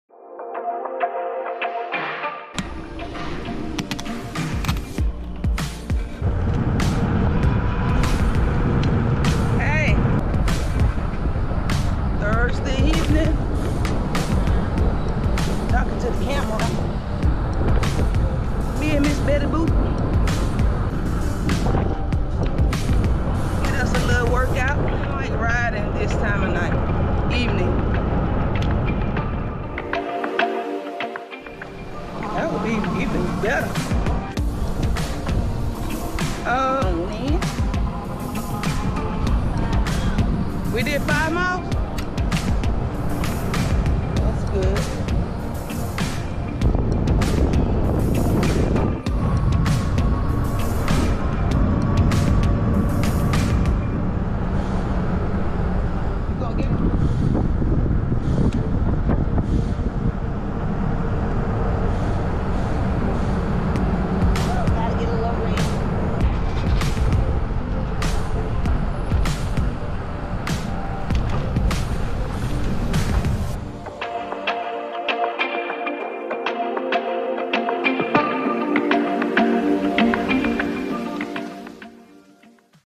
We Love Riding Our Lectric XPs
Seven miles of fun and exercise this evening with MsBettyBoop.